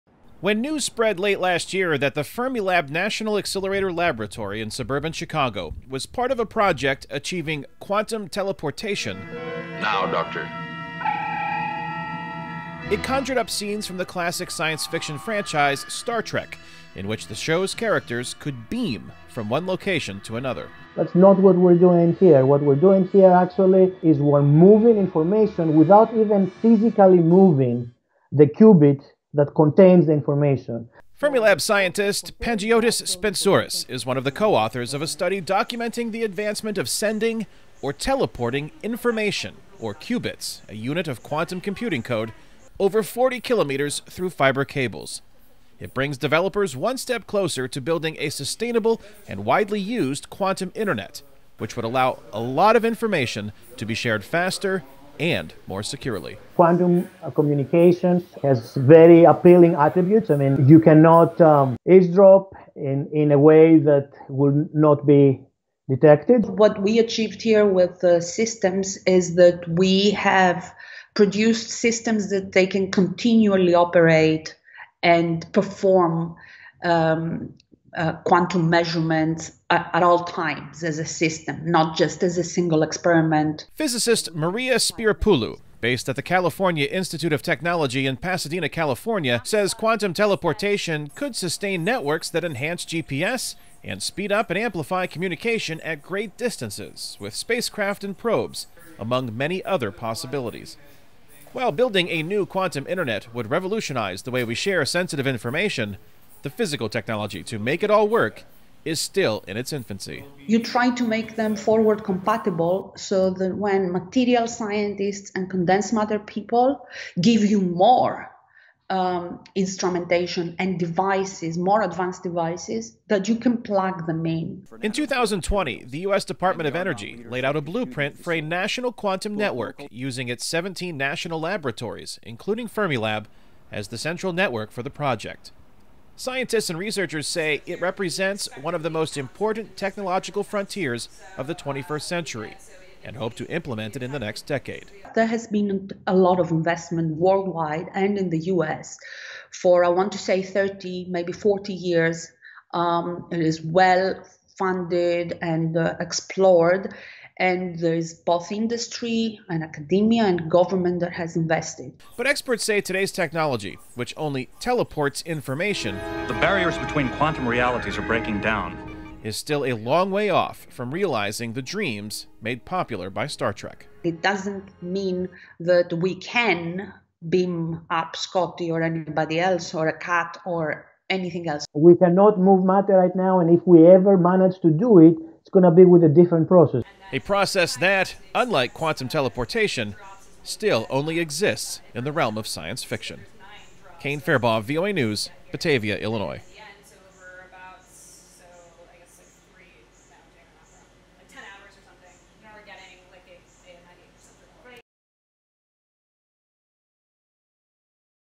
US Labs Paving Way for Quantum Internet
A breakthrough in advanced quantum technology frequently compared to fictional elements of popular science fiction show 'Star Trek' could revolutionize everyday life around globe